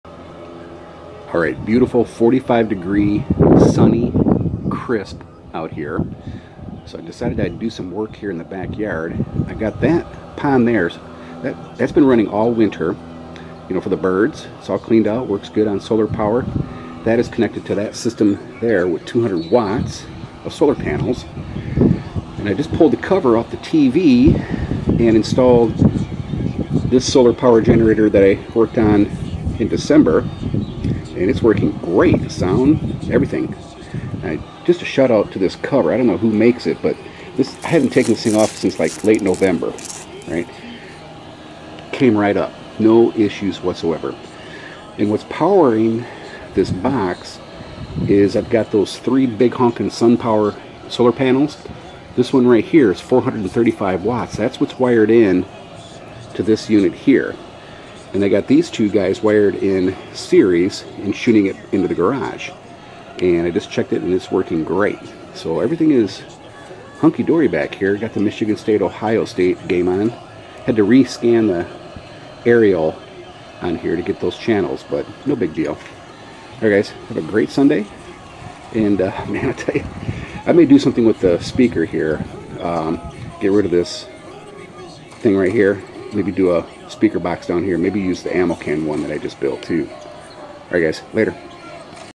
New 12v solar power generator installed & tested in the backyard patio
435w solar panel is powering this unit. This DIY homemade solar power generator replaces the two ammo cans I had there last year. I combined two Interstate Batteries (35aH each) into one watertight box. The solar charger is a EPEver Tracer 20a MPPT with MT50 meter. The inverter is a Aims Power 300w pure sine wave unit.

Chris DIYer Amazon Store: